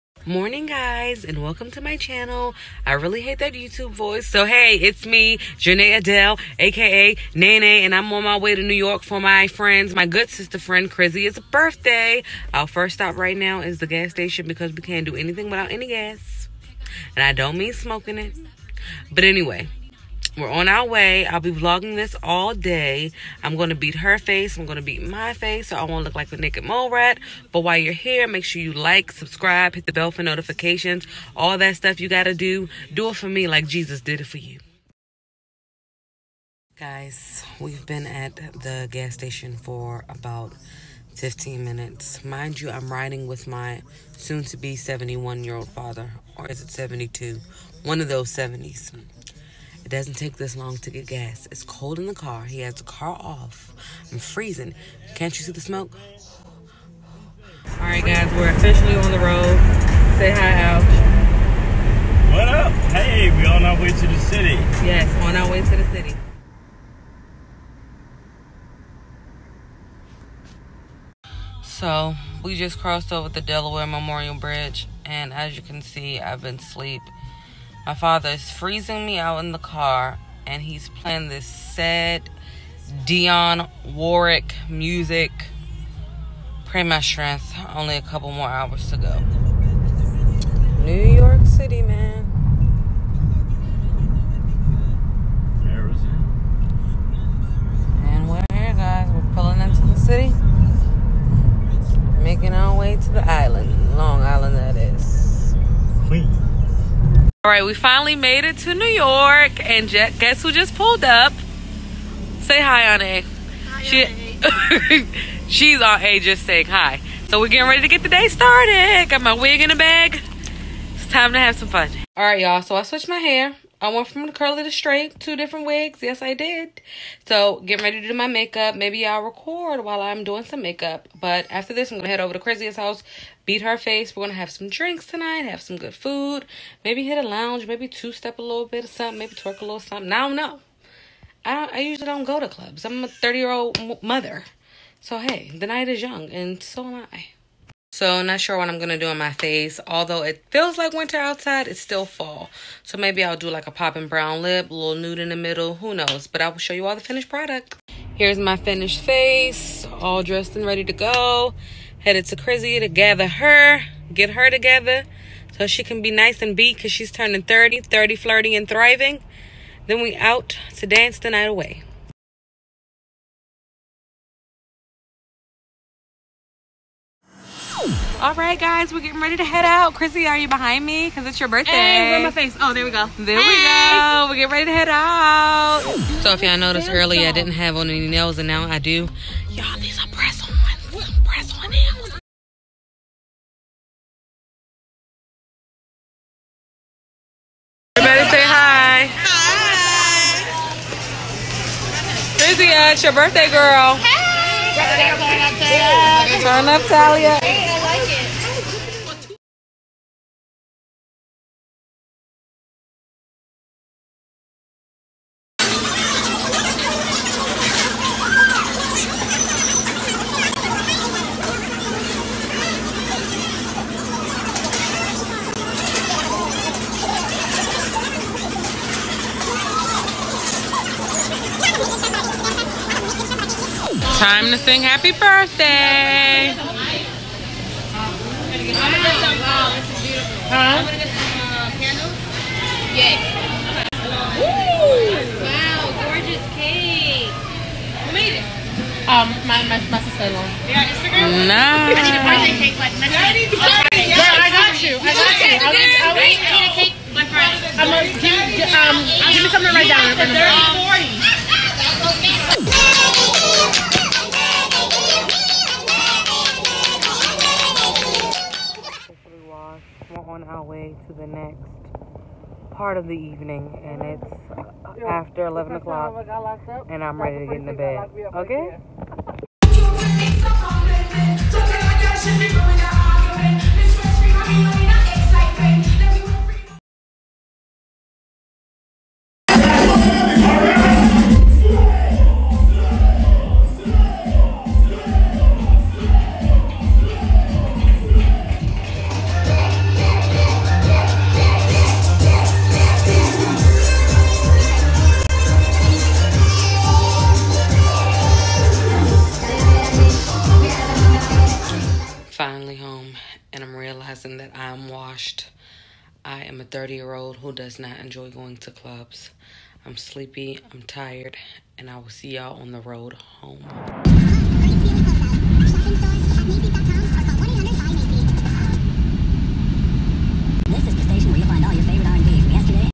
Me, My Press On Nails, and my Wig take NYC
Tried to hang and I couldn't! My Daddy is a trip and he froze me out! Yes, I wear press-ons and wigs! Happy Bday Krizia!